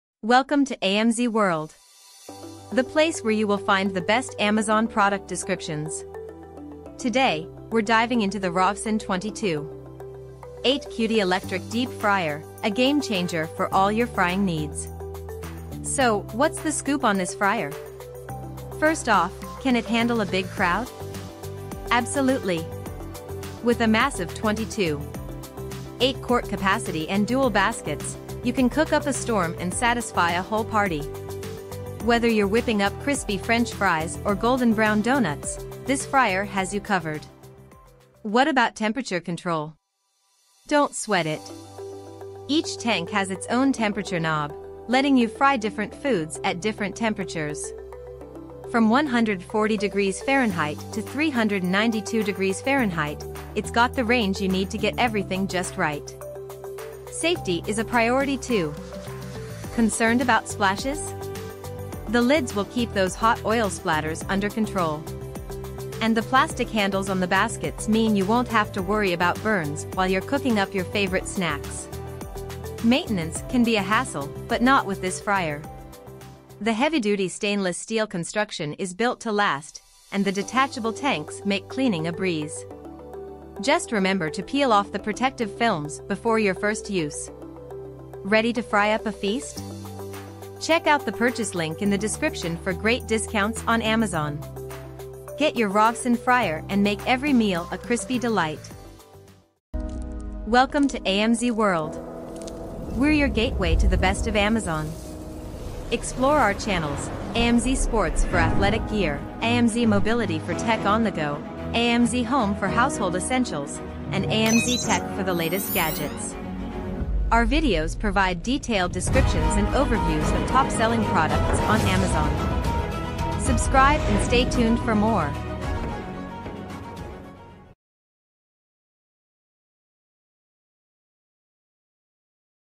Describing ROVSUN 22.8QT Electric Deep Fryer w/ 2 Baskets & Lids, 21.8L, Amazon
The ROVSUN 22.8QT Electric Deep Fryer seems like a solid choice if you’re looking for a versatile and high-capacity fryer, especially useful for commercial settings or larger gatherings. Here’s a breakdown of its key features:

Key Features:
Dual Fryer Baskets: With two separate tanks, you can cook different foods simultaneously at different temperatures, making it ideal for busy kitchens or varied menu items.
Capacity: Each basket holds up to 4 liters (approximately 4.2 quarts), with a total oil capacity of 13.7 quarts (12.96 liters), providing ample space for large quantities.
Temperature Control: Individual temperature knobs for each tank allow precise control over cooking conditions, with a range from 60°C to 200°C (140°F to 392°F).
Safety Features: Includes lids to prevent splashing and plastic handles on the baskets to avoid burns. The temperature indicator light helps monitor heating.
Durability & Maintenance: Made from heavy-duty stainless steel, it’s built to last. The tanks can be detached for easy cleaning, though you’ll need to peel off protective films before use.
Considerations:
Power Requirement: Requires a 16A outlet, which is higher than standard outlets, so you may need to ensure your electrical setup can handle it.
Size & Weight: It’s fairly large (16.9"D x 21.25"W x 11.8"H) and weighs around 7 kg, so make sure you have enough counter space for it.
Overall, this fryer seems to offer a good combination of capacity, control, and durability, making it a versatile tool for frying a variety of foods.

Welcome to AMZ World! The place where you will find the best Amazon product descriptions! Today, we’re diving into the ROVSUN 22.8QT Electric Deep Fryer—a game changer for all your frying needs.

So, what’s the scoop on this fryer? First off, can it handle a big crowd? Absolutely! With a massive 22.8-quart capacity and dual baskets, you can cook up a storm and satisfy a whole party. Whether you're whipping up crispy French fries or golden-brown donuts, this fryer has you covered.

What about temperature control? Don’t sweat it! Each tank has its own temperature knob, letting you fry different foods at different temperatures. From 140°F to 392°F, it’s got the range you need to get everything just right.

Safety is a priority too! Concerned about splashes? The lids will keep those hot oil splatters under control. And the plastic handles on the baskets mean you won’t have to worry about burns while you’re cooking up your favorite snacks.

Maintenance can be a hassle, but not with this fryer. The heavy-duty stainless steel construction is built to last, and the detachable tanks make cleaning a breeze. Just remember to peel off the protective films before your first use.

Ready to fry up a feast? Get your ROVSUN fryer and make every meal a crispy delight!